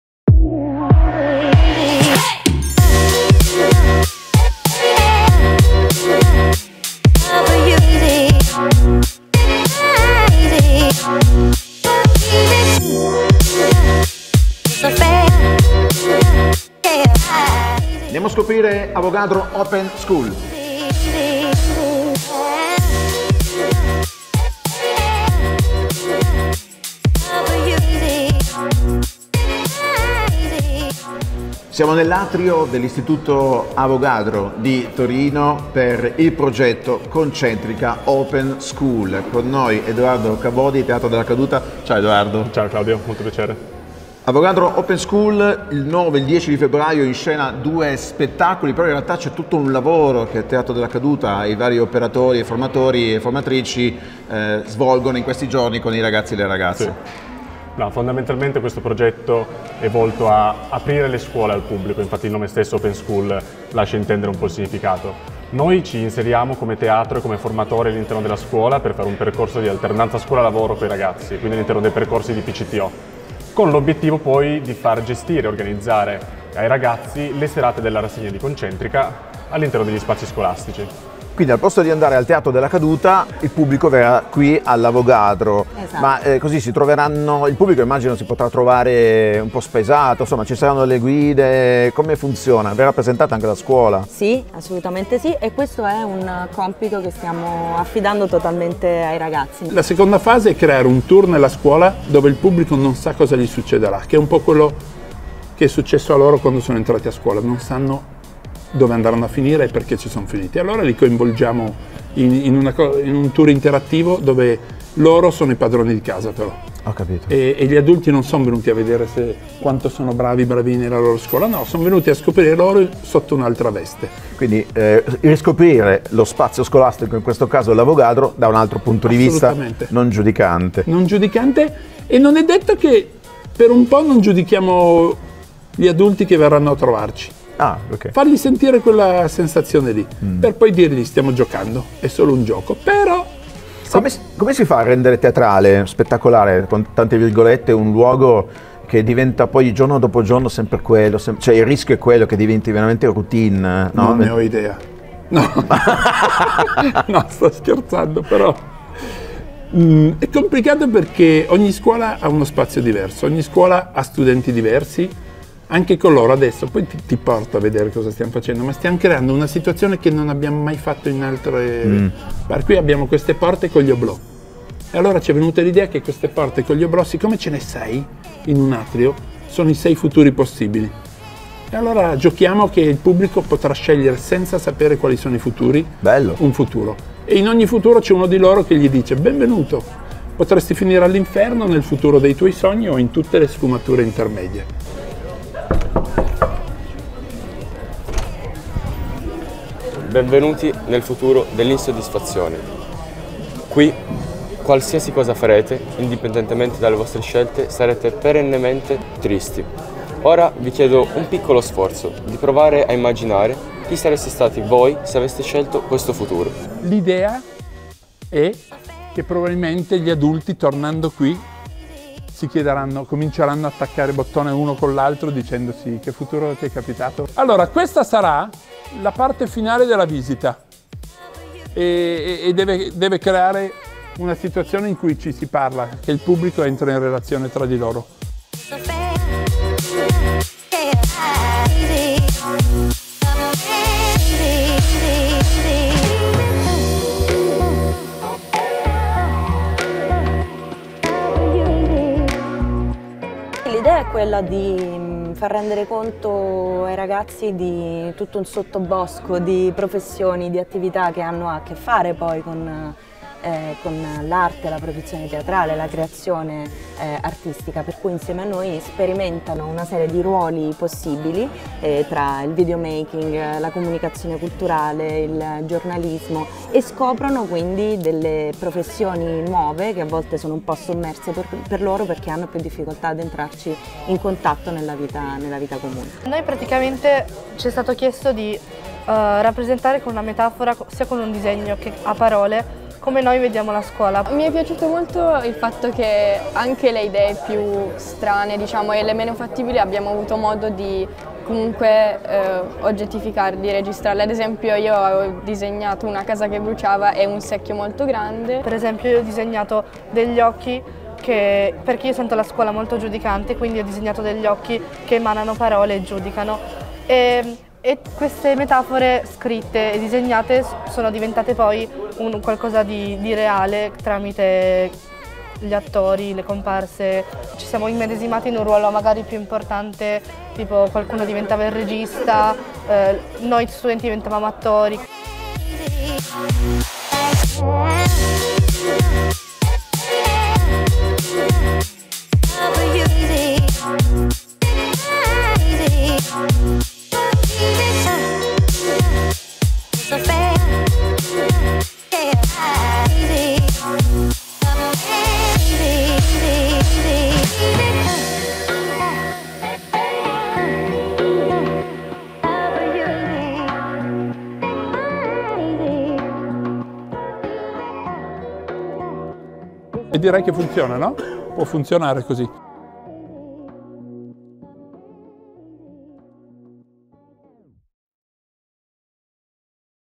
Siamo andati a scoprire Concentrica Open School all'Avogadro di Torino
Siamo stati nelle aule dell'Istituto Avogadro di Torino per scoprire il dietro le quinte di Concentrica Open School. Si tratta di un progetto di Teatro della Caduta e Torino Open Lab che nel mese di febbraio 2023 ha coinvolto gli studenti della storica scuola torinese.

Con Open School la rassegna Concentrica apre agli spettatori le porte di istituti superiori torinesi con aperitivi, tour guidati e spettacoli organizzati dagli stessi studenti che li frequentano. Questi eventi sono il risultato di un progetto più ampio che ha un duplice scopo: portare il mondo del teatro agli studenti delle scuole superiori e aprire gli istituti alla cittadinanza, permettendo al pubblico esterno di partecipare agli spettacoli serali.

Servizio di Claudio Petronella